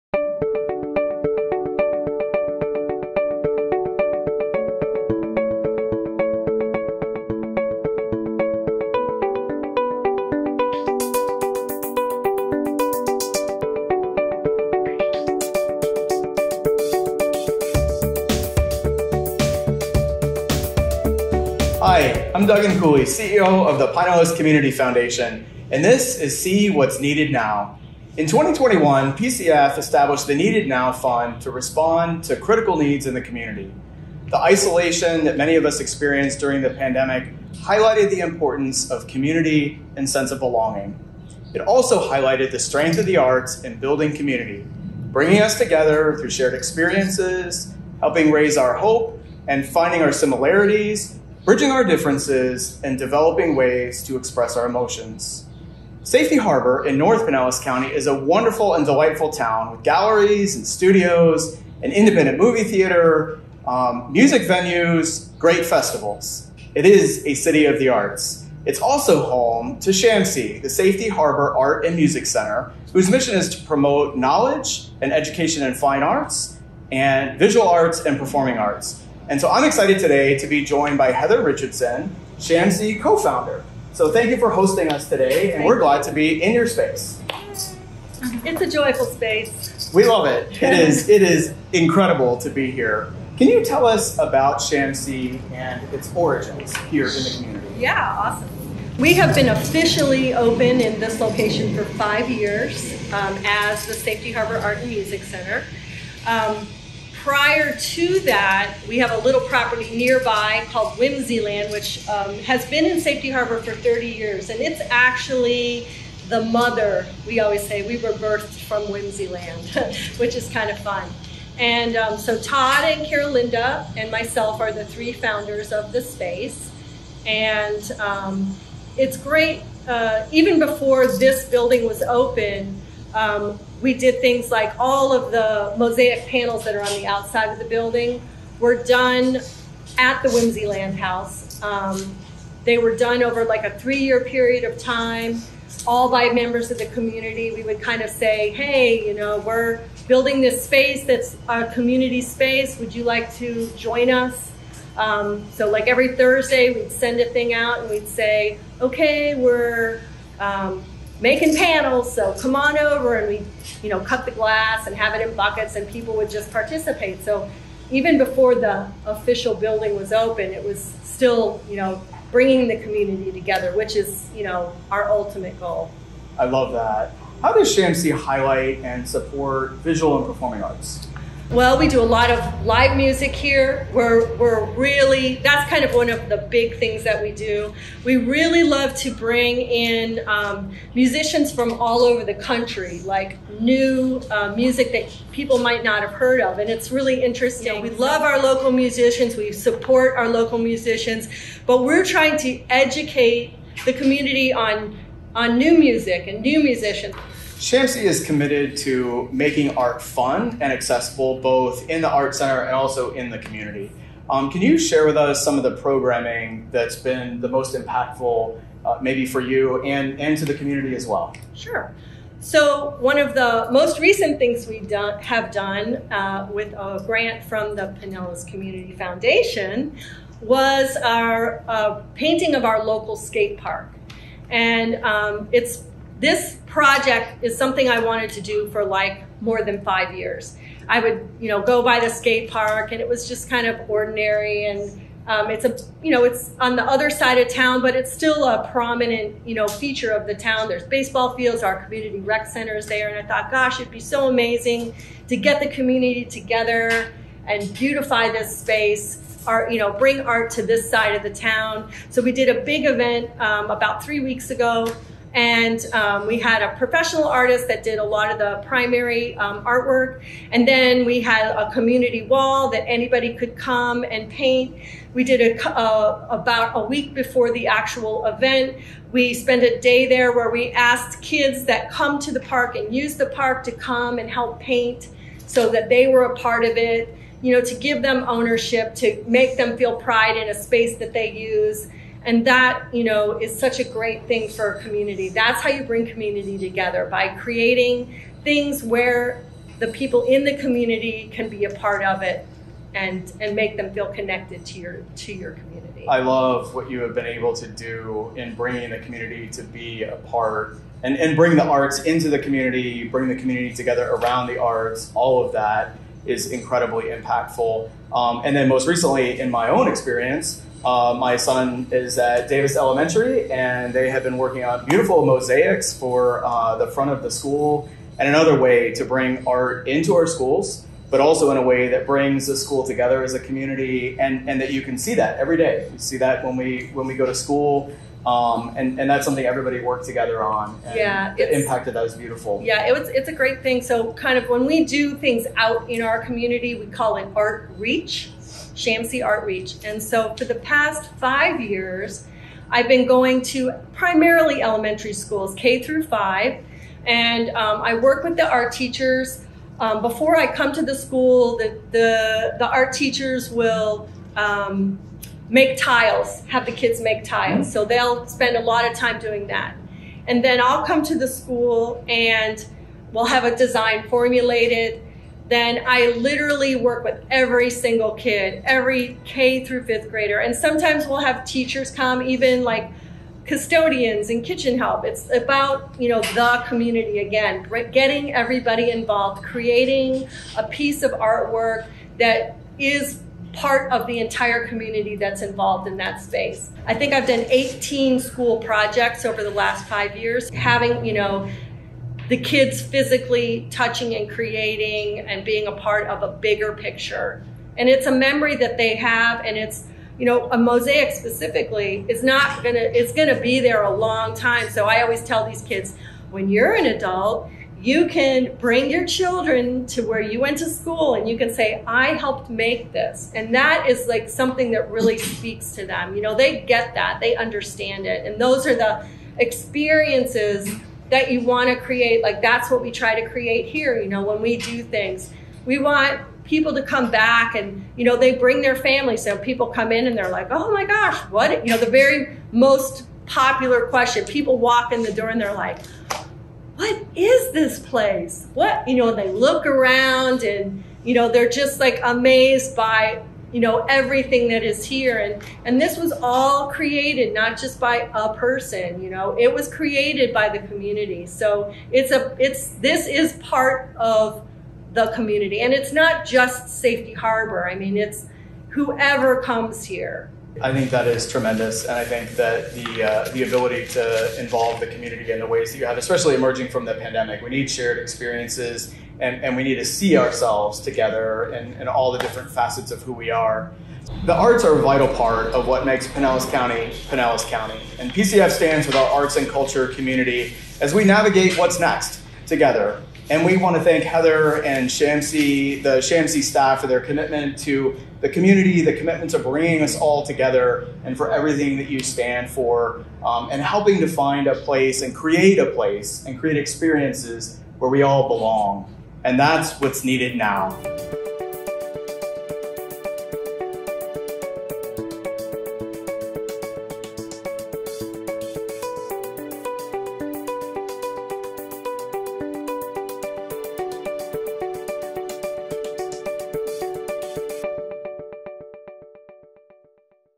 SHAMC - Needed Now Interview
Join Duggan Cooley, CEO of the Pinellas Community Foundation, as he interviews Heather Richardson from the Safety Harbor Arts and Music Center and discusses the importance of accessible arts experiences for our community. For more information about how you can contribute to the sustainability of the arts and culture community in Pinellas County,